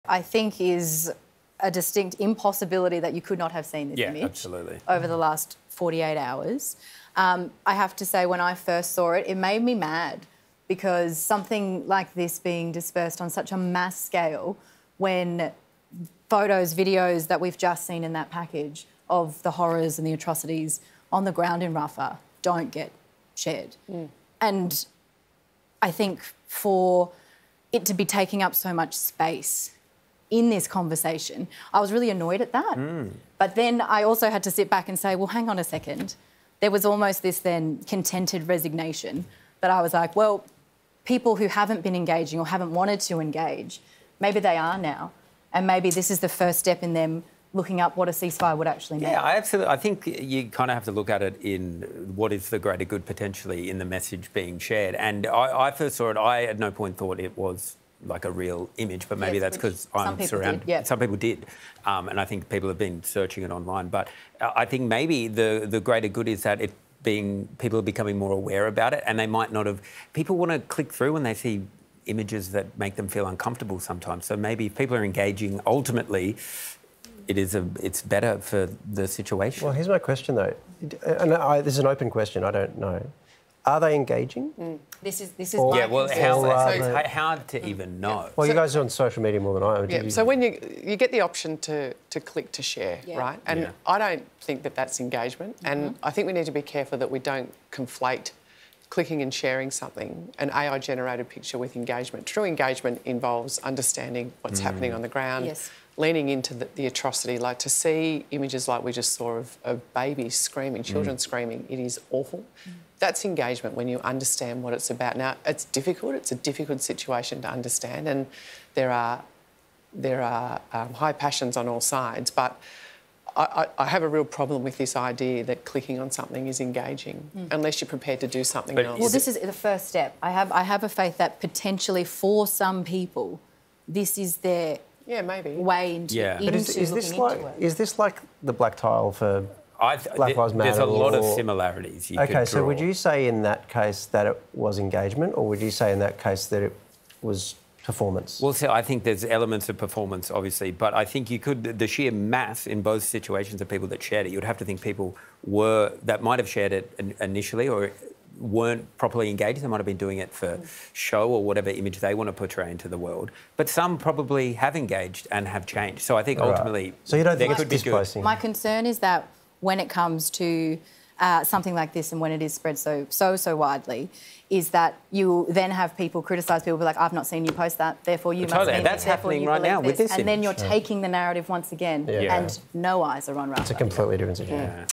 The Debate Over Whether The Sharing Of The ‘All Eyes On Rafah’ Viral Image Is Performative
The debate over whether the sharing of the ‘All Eyes On Rafah’ viral image is performative or meaningful.